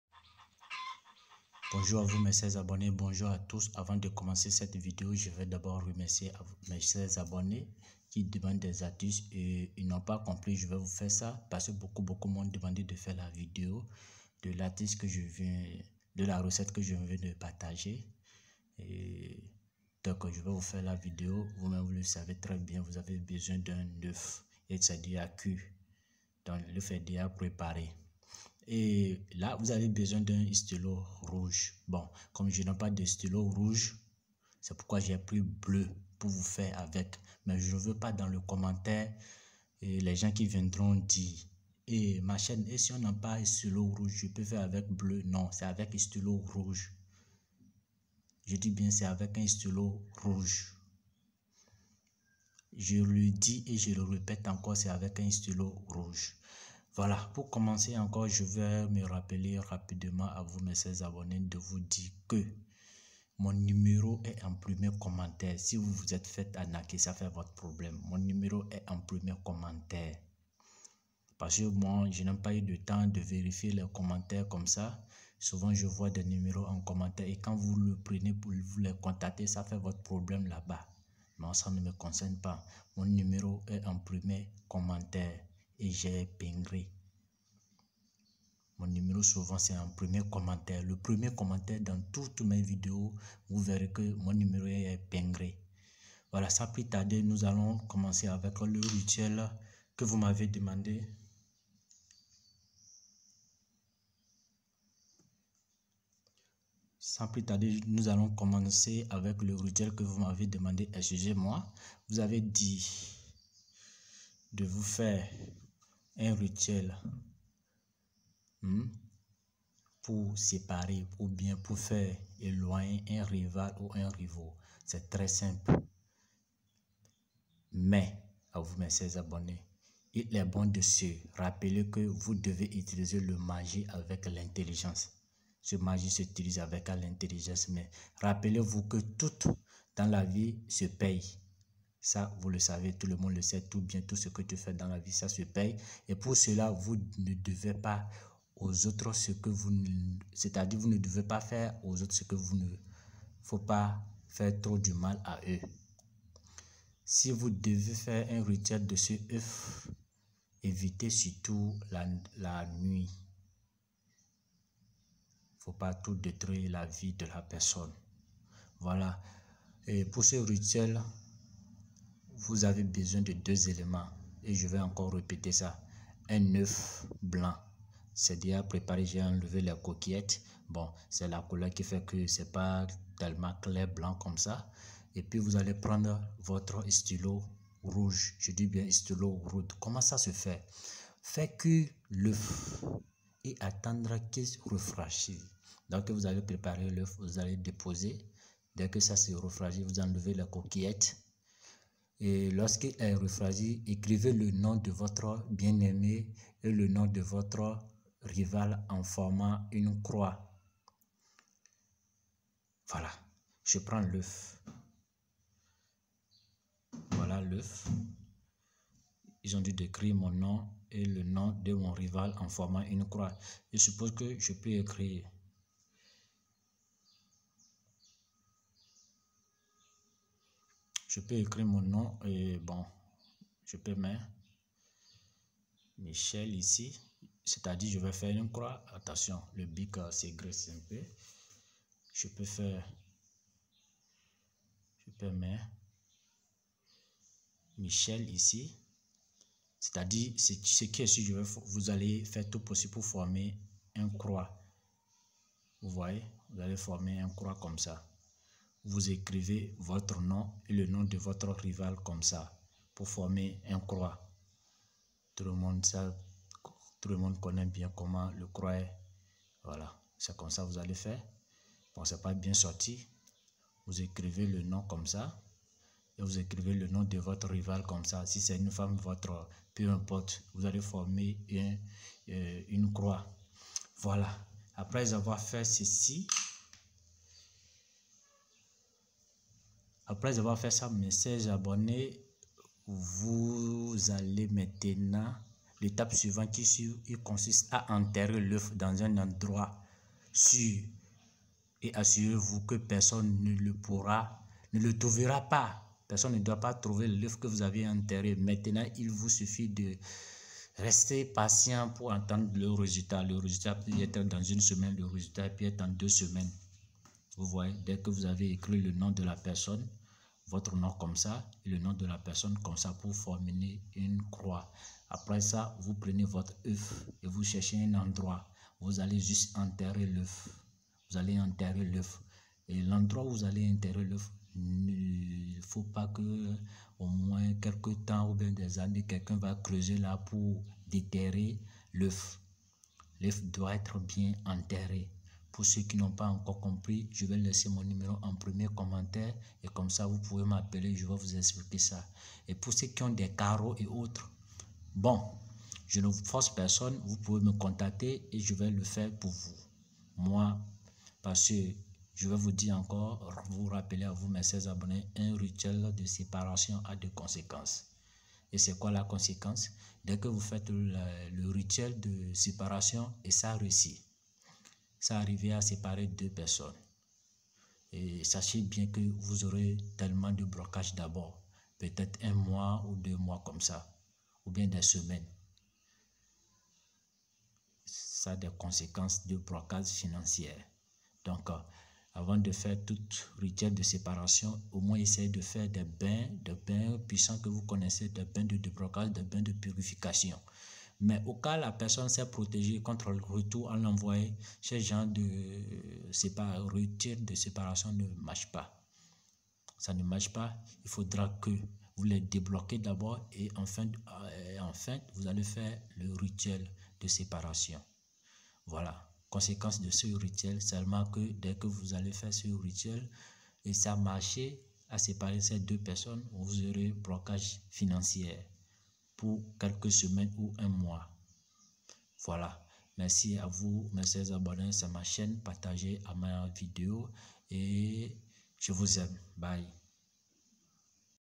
RITUEL D'OEUF POUR ÉLOIGNER VOTRE RIVAL
Rituel d’œuf pour éloigner votre rival

Il est bon de se rappeler que vous devez utiliser la magie avec intelligence. Rappelez-vous que tout dans la vie se paie et pour cela vous ne devez pas faire aux autres ce que vous n’aimeriez pas qu’on vous fasse. Cela dit, si vous devez faire un rituel d’œuf, évitez surtout de nuire à la vie d’autrui.

Matériaux nécéssaires

1 blanc d’œuf

1 stylo rouge

Mode d’emploi

Faire cuire l’œuf et attendre qu’il refroidisse. Lorsqu’il est refroidi, écrivez le nom de votre bien-aimé et le nom de votre rival, en formant une croix. L’étape suivante consiste à enterrer l’œuf dans un endroit sûr et assurez-vous que personne ne le trouvera.
Maintenant, il vous suffit de rester patient et attendre le résultats💔